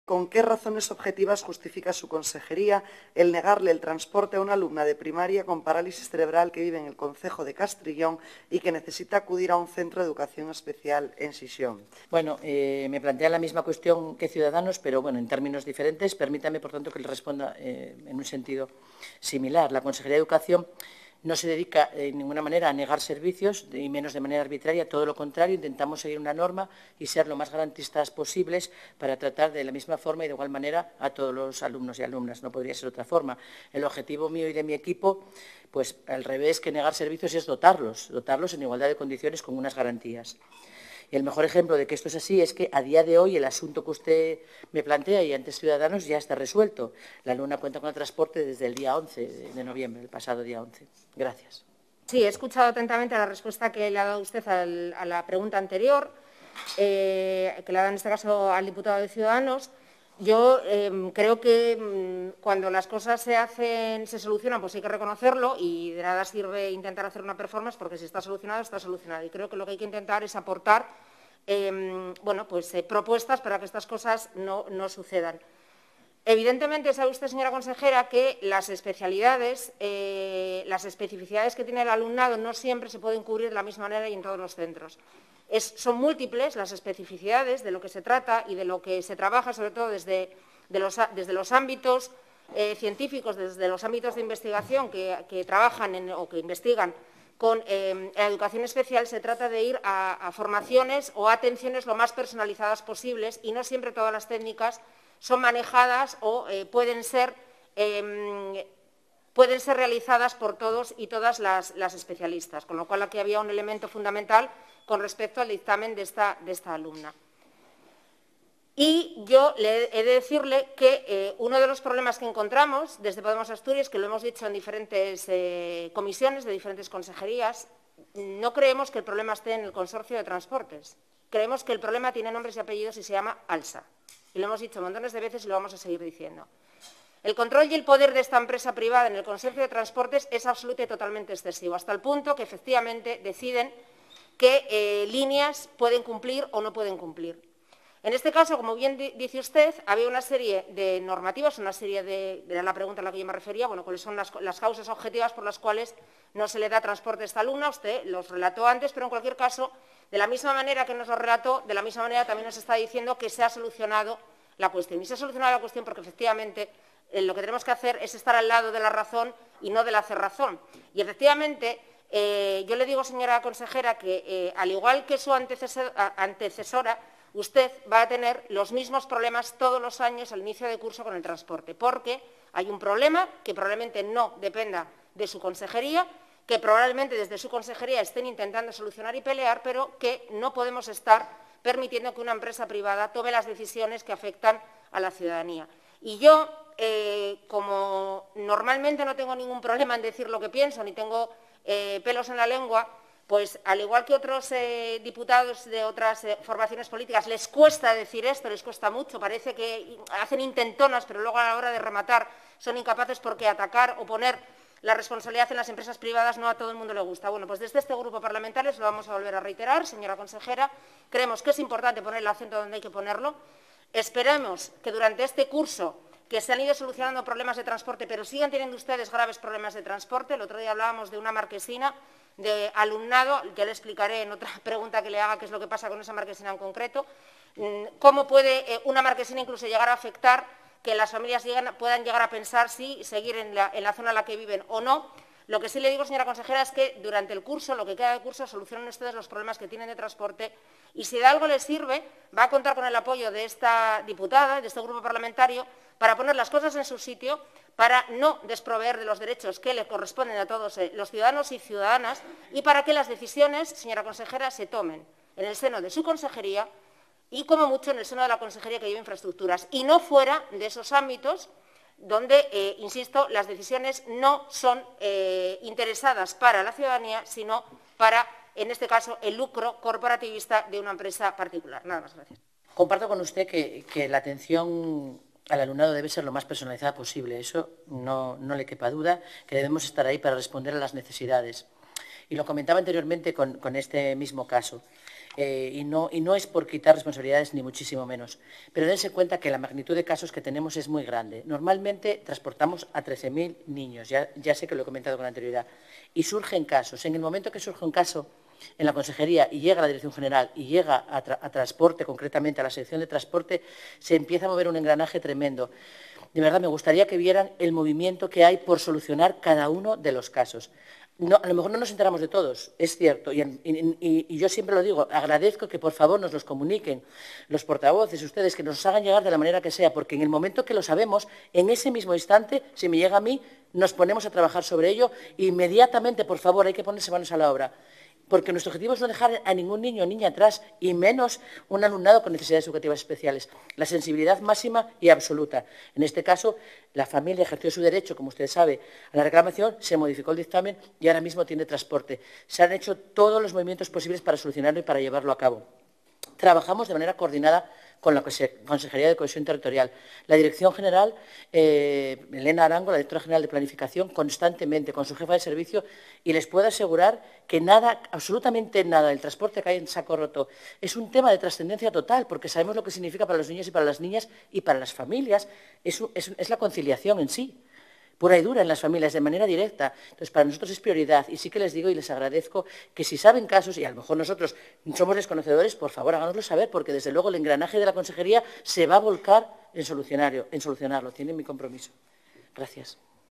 Pq niegan el transporte a una alumna con parálisis cerebral al centro de Educación Especial en Xixón
211118 Comisión de Educación